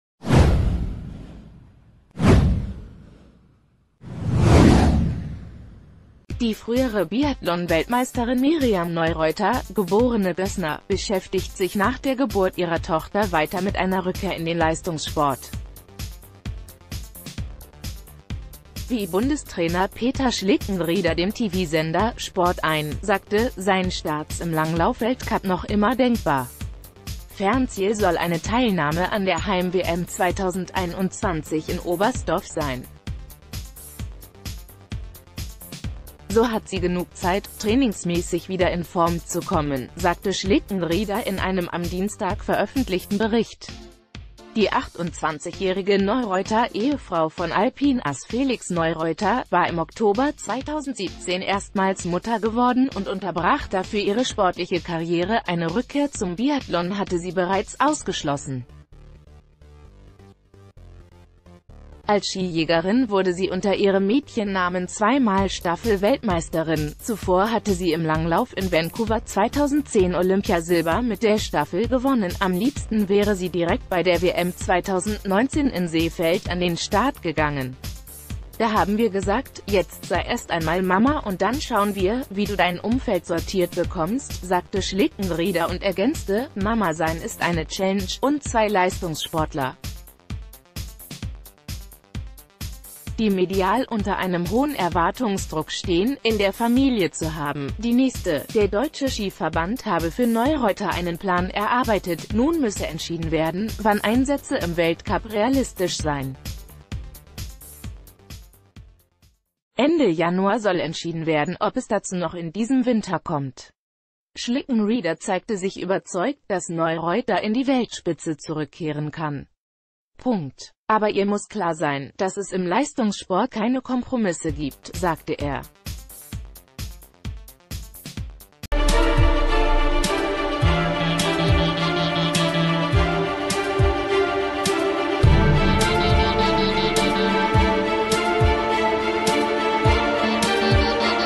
Biathlon: Miriam Neureuther plant Comeback im Langlauf-Weltcup
Die frühere Biathlon-Weltmeisterin Miriam Neureuther, geborene Gössner, beschäftigt sich nach der Geburt ihrer Tochter weiter mit einer Rückkehr in den Leistungssport. Wie Bundestrainer Peter Schlickenrieder dem TV-Sender "Sport1" sagte, seien Starts im Langlauf-Weltcup noch immer denkbar. Fernziel soll eine Teilnahme an der Heim-WM 2021 in Oberstdorf sein. "So hat sie genug Zeit, trainingsmäßig wieder in Form zu kommen", sagte Schlickenrieder in einem am Dienstag veröffentlichten Bericht. Die 2...